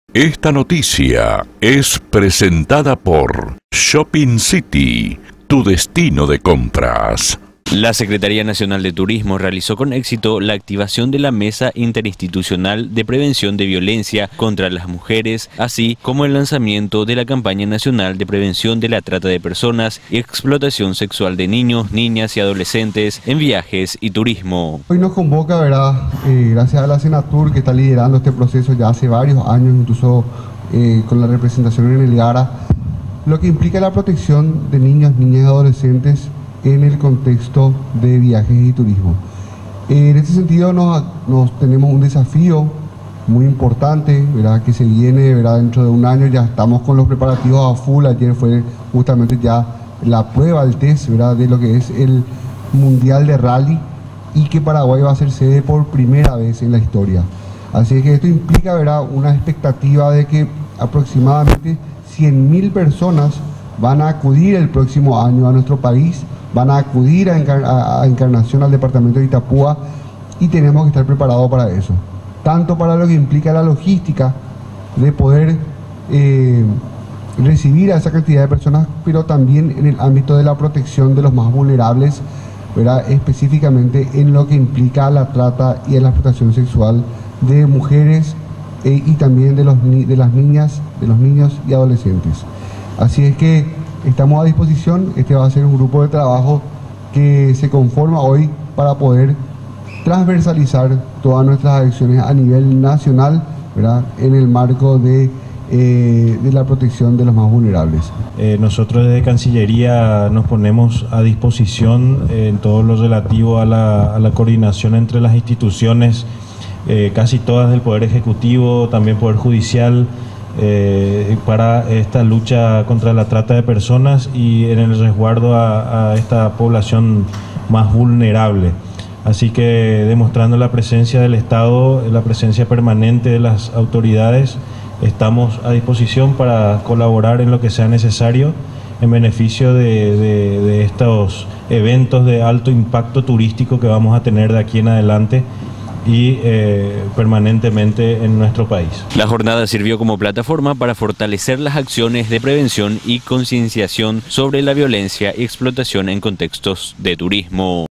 Éxito en la Activación de la Mesa Interinstitucional de Prevención de Violencia contra las Mujeres
Lanzamiento de campaña nacional contra la trata de personas y explotación sexual en el marco del evento automovilístico.